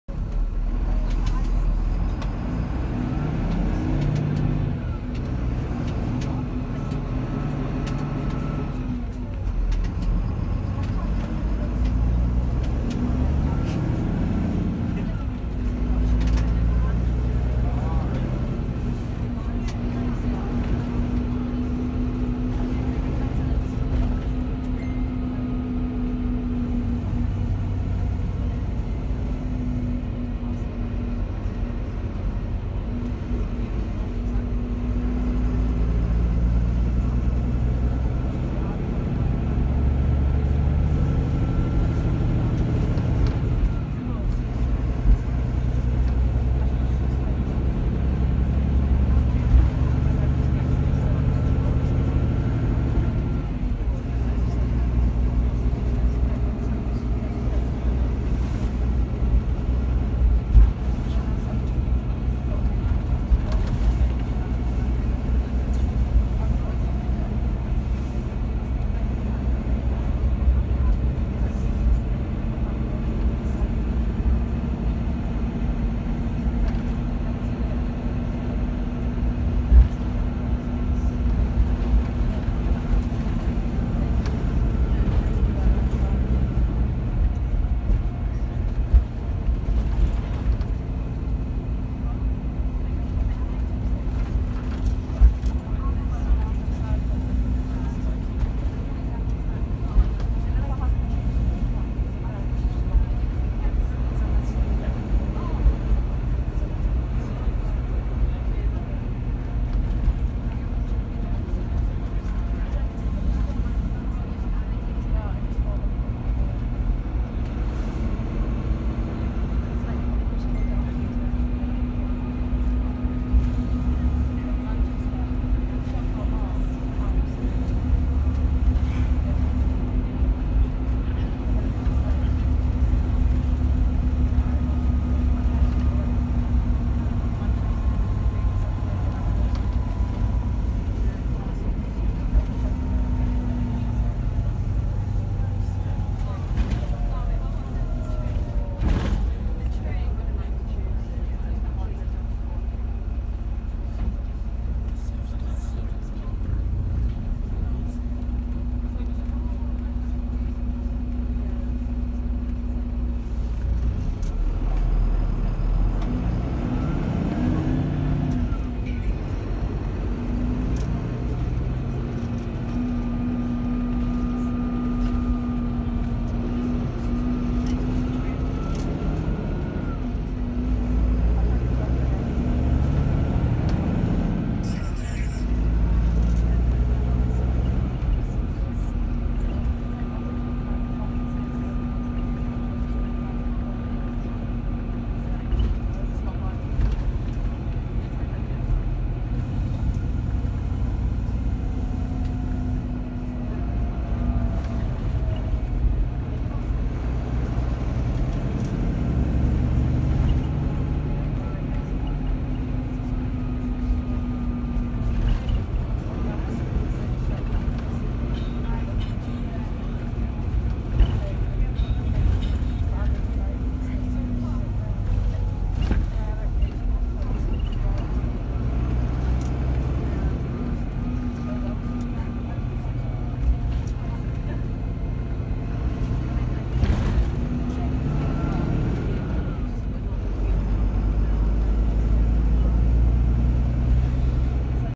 Rail Replacement: Spencer Graham Coaches FJ11 MMU Volvo B9R Caetano Levante
Operating A Rail Replacement Service towards Hexham from Newcastle.Enjoy!

Date Recorded: **/02/2022

Transport in the UK
Tees Buses
Ryan’s Transport Videos
Northern Transport
County Durham Buses